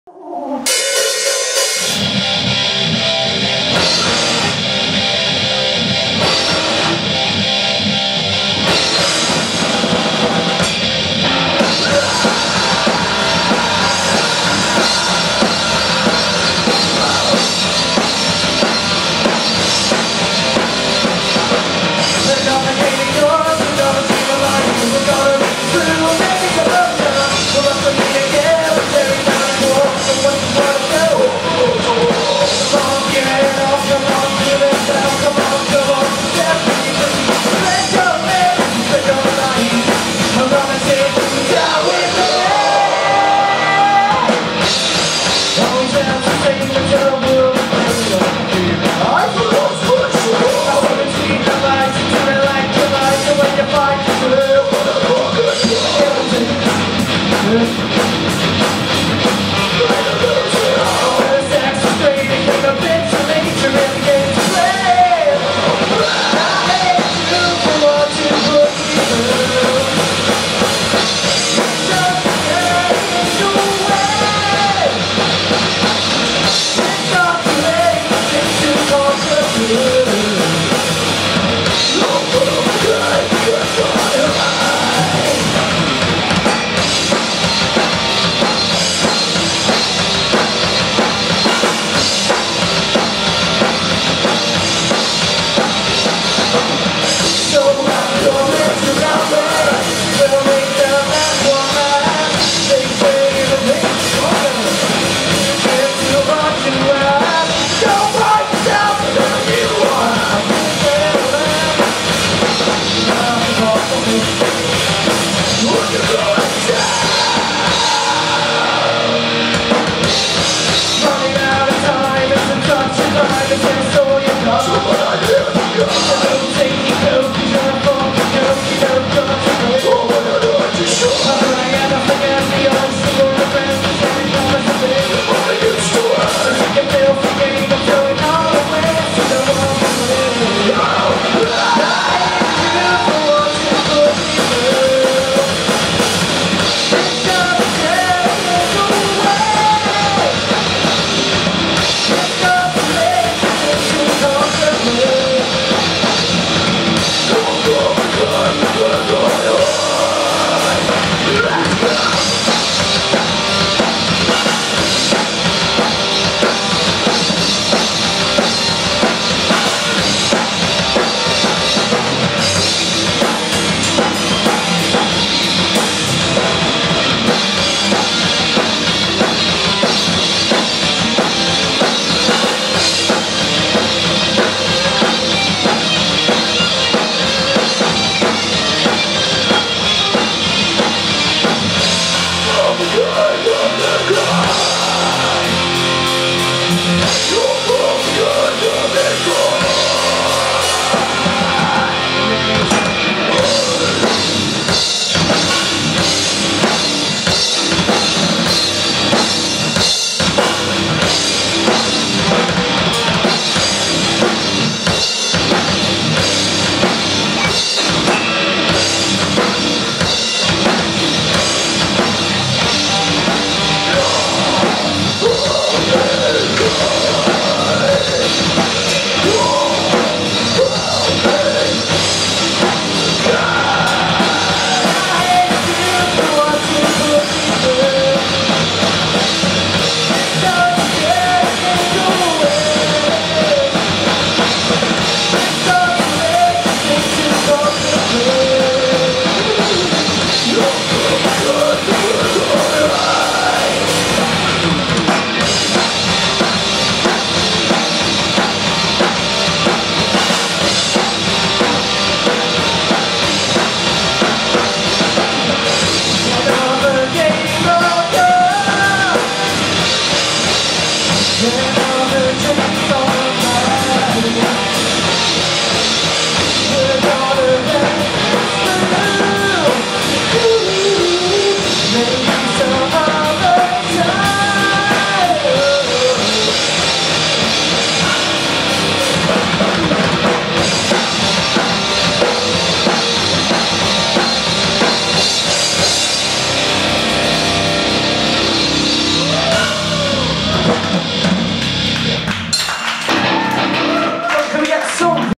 Behead the Bride at The Green Star
Recorded at the Green Star,Smallthorne 18th August 2013.